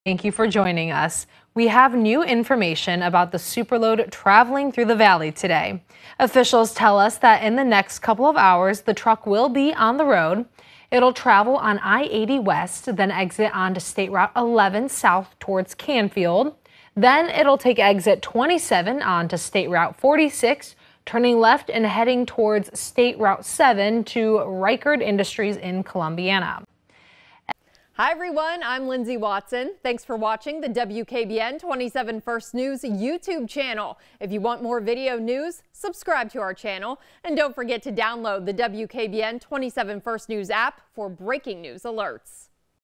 150-ton superload truck to stall traffic in the area
Superload truck to stall traffic in Mercer, Columbiana counties